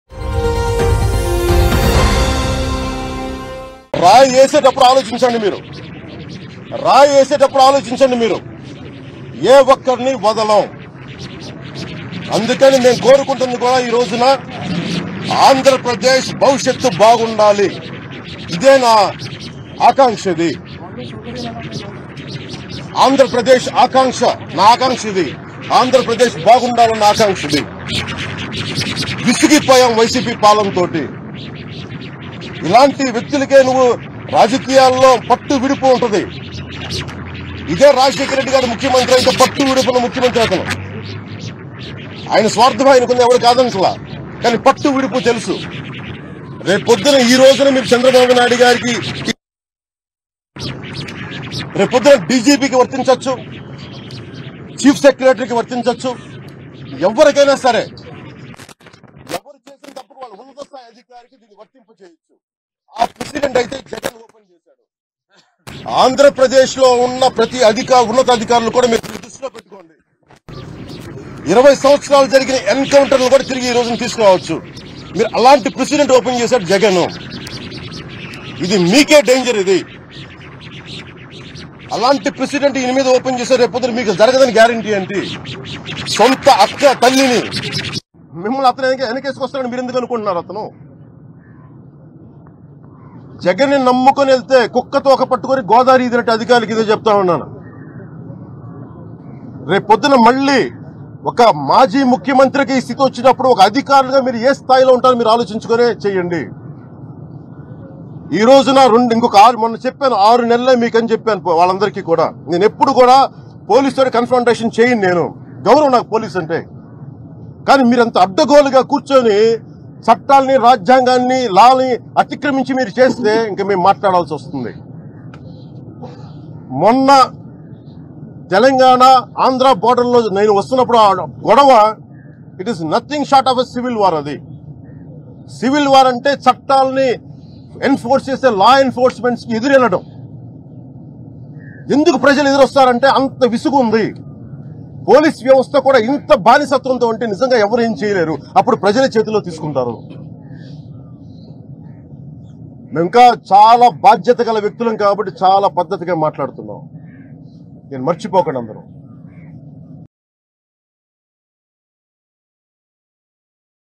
వైఎస్‌ స్వార్థపరుడే.. డీజీపీకి పవన్‌ స్ట్రాంగ్‌ వార్నింగ్ || Pawan Comments On YS Jagan || Dial News
వైఎస్‌ స్వార్థపరుడే.. డీజీపీకి పవన్‌ స్ట్రాంగ్‌ వార్నింగ్ || Pawan Kalyan Comments On YS Jagan || Dial News